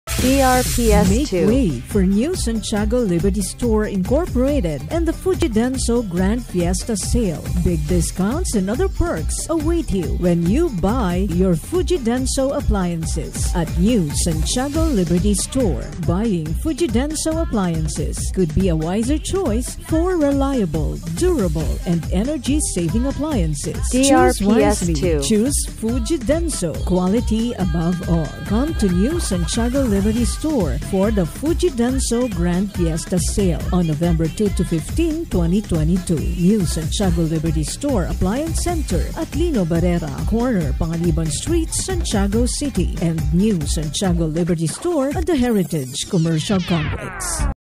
New Santiago Liberty Store Fujidenzo Promo Radio Ad November 2022 47s (Santiago City, Isabela)
This is the November 2022 Provincial Radio Ad for New Santiago Liberty Store promoting their Fujidenzo Promo.

DRPS Two 2741: New Santiago Liberty Store Fujidenzo Promo Radio Ad November 2022 47s (Santiago City, Isabela)